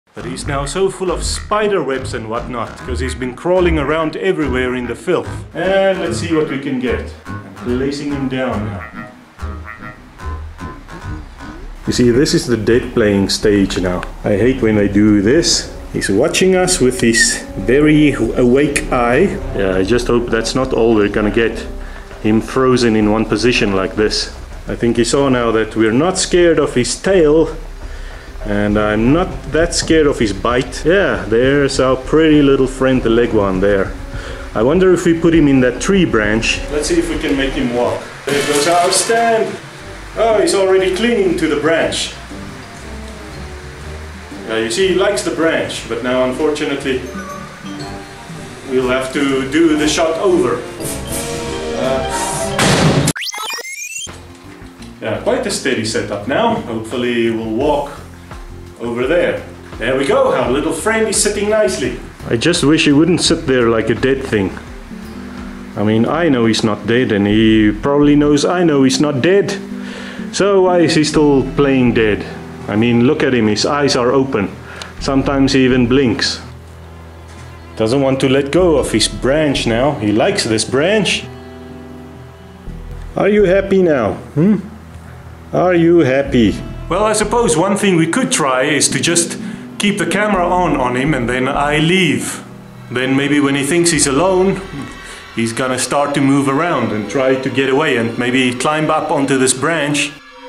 LIKKEWAAN rock monitor lizard PLAYS DEAD during film shoot
I try to shoot a leguaan on a greenscreen for use in movies, but unfortunately as lively and aggressive as this fella was just minutes before, it seems he's either camera shy or playing dead, and has no intentions of moving around.